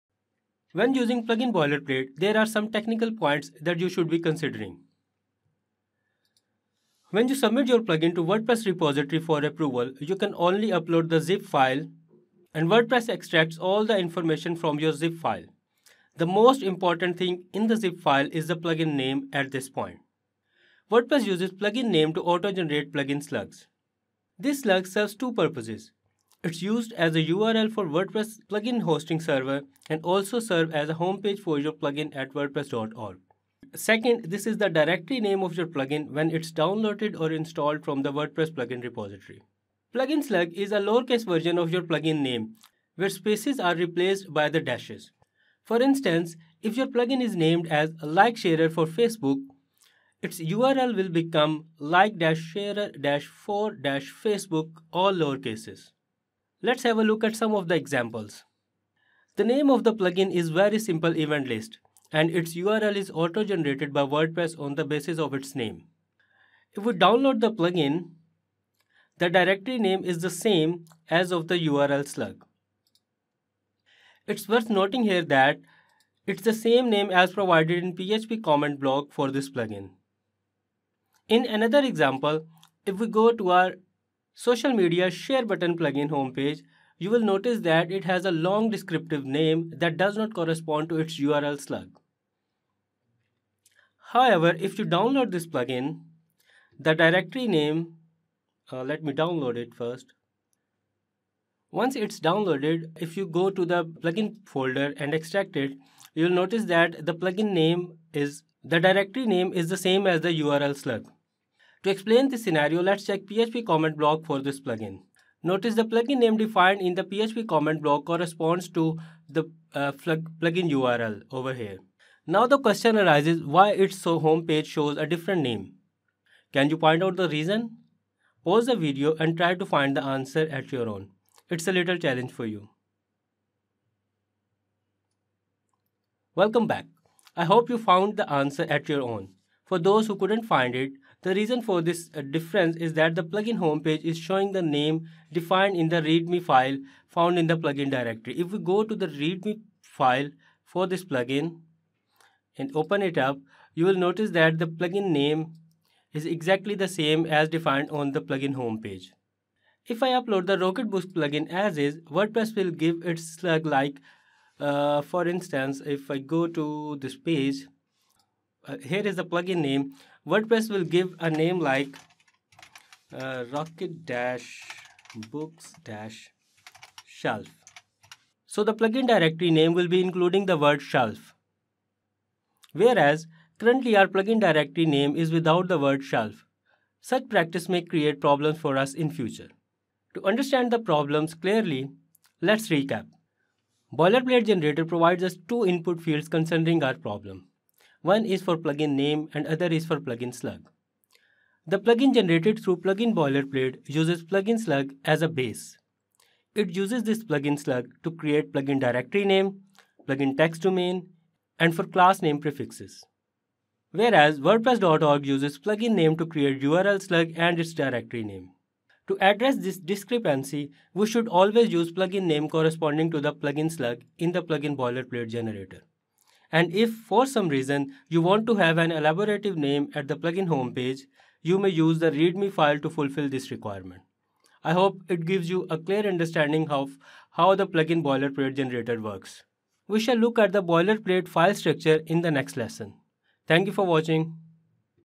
WP Plugin Development using Boilerplate : Tips for using Boilerplate Generator
In this Video, we look at some of the things to consider while using online WordPress Plugin Boilerplate generator.

We shall see how filling in different fields will affect the plugin on Wordpress.org repo.